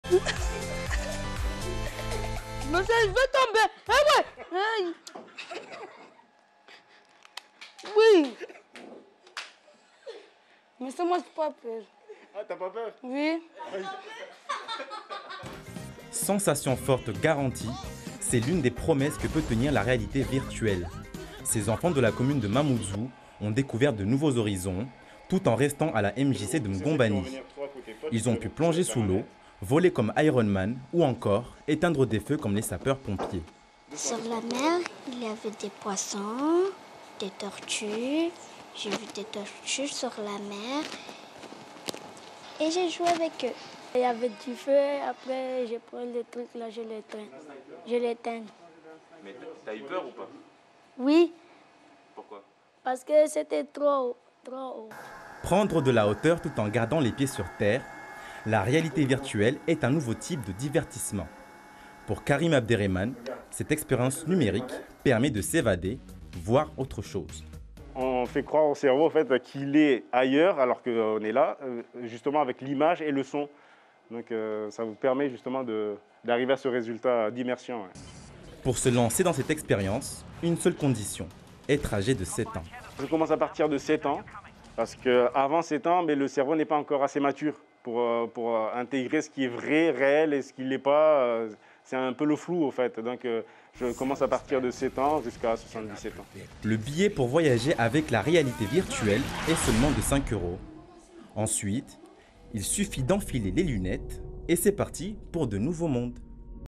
Voyager grâce à la réalité virtuelle
Voyager grâce à la réalité virtuelle : Une première à Mayotte, KuléVR propose de partir à la découverte du monde par la biais de la réalité virtuelle.